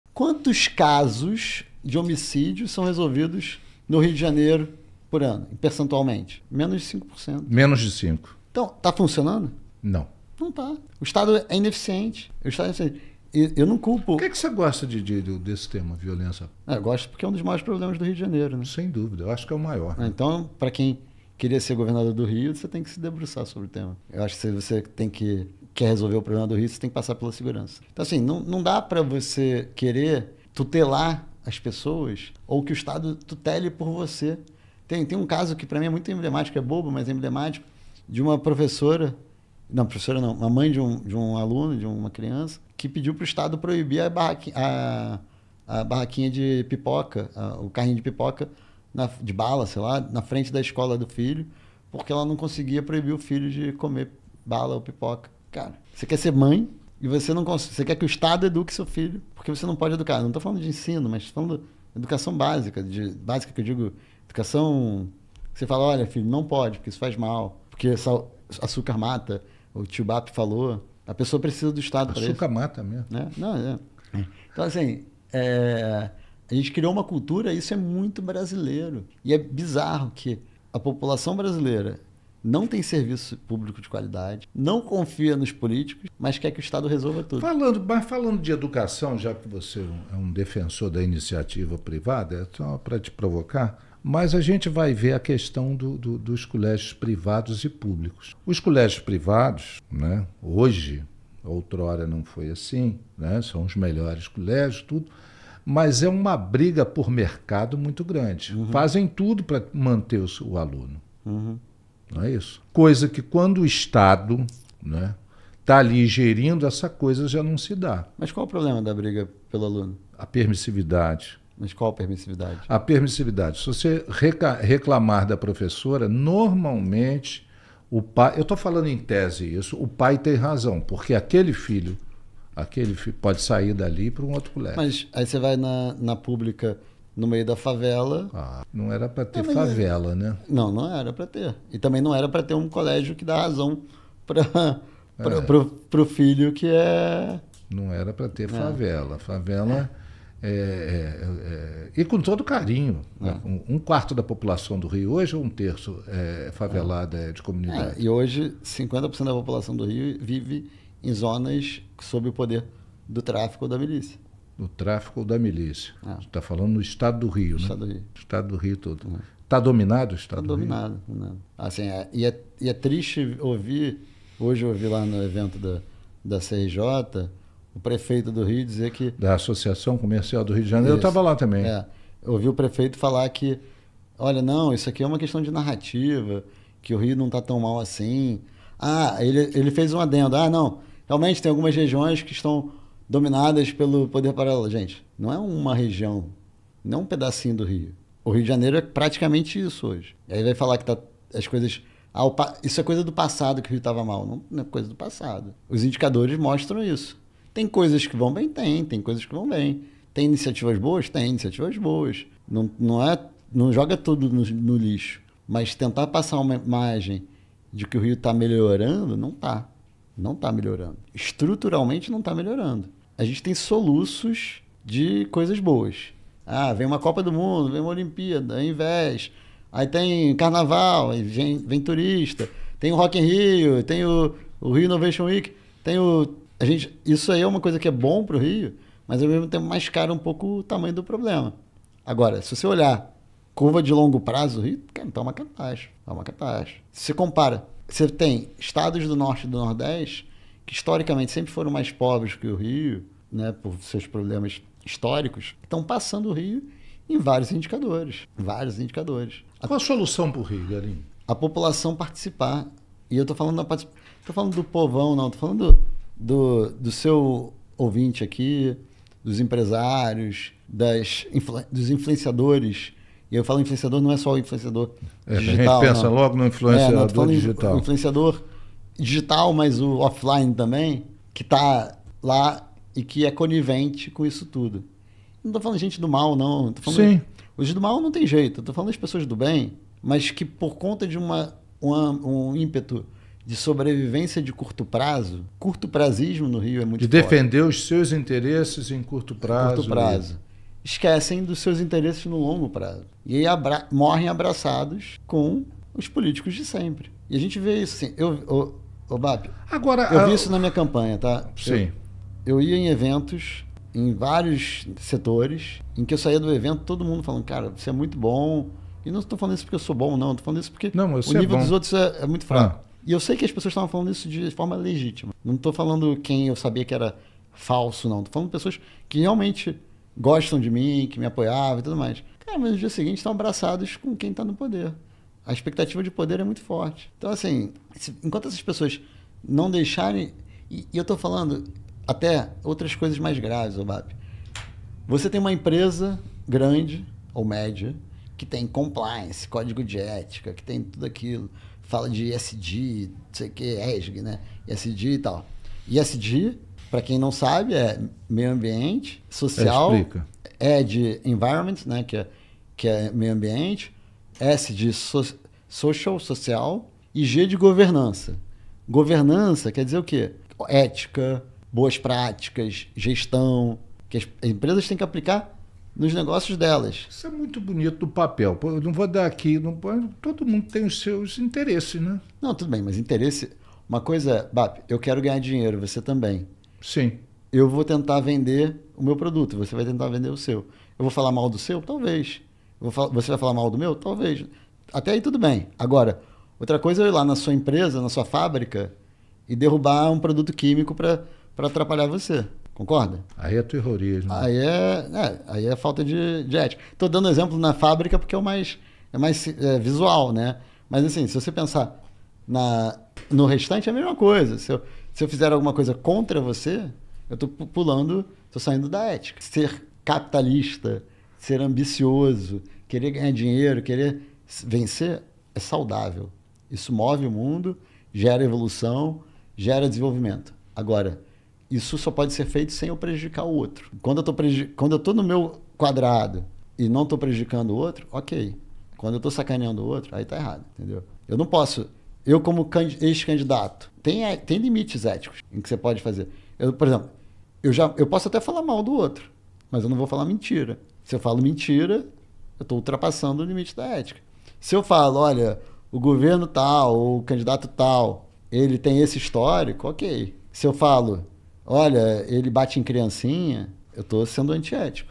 Estado resolve tudo, Política no Rio e ESG com comentários de Paulo Ganime em Podcast
Quem é Paulo Ganime?

Paulo Ganime tem 40 anos, é Engenheiro de Produção pelo CEFET-RJ, além de ter feito Economia na UERJ e um MBA na PUC-RIO. Trabalhou 15 anos em grandes empresas multinacionais nas áreas financeiras e de gestão de projetos, sendo os últimos 5 desses como expatriado na França e nos EUA.
Paulo é carioca e sempre amou e defendeu muito o Rio e o Brasil. A distância do Brasil só fez aumentar sua vontade de querer fazer algo pelo país. Quando o Novo foi registrado em 2015, ele se filiou de cara, tamanha a identificação com seus ideais. em 2017, essa vontade de ajudar o Brasil falou mais alto. Paulo se inscreveu no processo seletivo do Novo para Deputado Federal, voltou para o Brasil e mudou sua trajetória profissional.